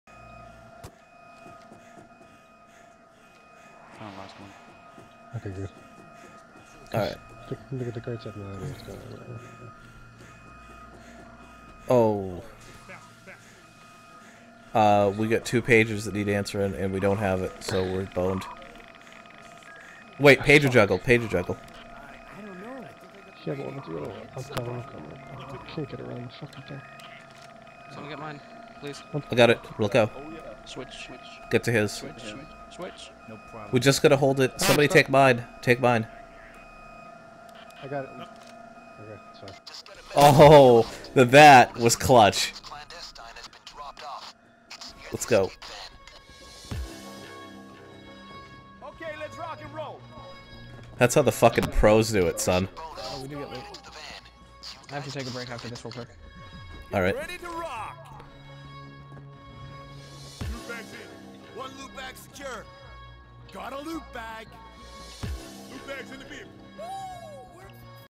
Never Give Up, Never Surrender
We need 20 seconds, but 2 pagers are going off and we only have 1 left.  What to do?  An impromptu pager juggle just long enough to trigger the escape.